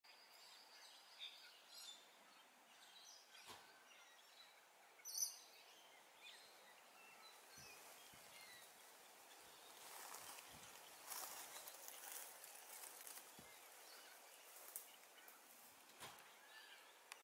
オーストラリアのライチョウバト Squatter Pigeon
ライチョウに似ている鳩というのでこういう和名が付いたんだと思います。
警戒心があまりない所はキジバトっぽいですね。